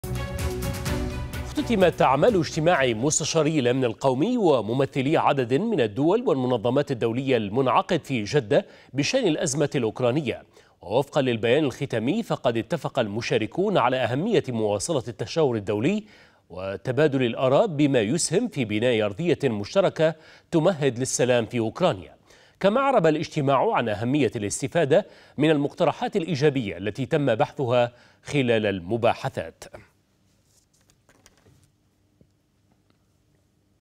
اختتام اجتماع مستشاري الأمن القومي بشأن الأزمة الأوكرانية في جدة
اختُتمت أعمال اجتماع مستشاري الأمن القومي وممثلي عدد من الدول، والمنظمات الدولية المنعقد في جدة بشأن الأزمة الأوكرانية. ووفقا للبيان الختامي، فقد اتفق المشاركون على أهمية مواصلة التشاور الدولي وتبادل الآراء بما يسهم في بناء أرضية مشتركة تمهد للسلام في أوكرانيا.
كما أعرب الاجتماع عن أهمية الاستفادة من المقترحات الإيجابية التي تم بحثها خلال المباحثات.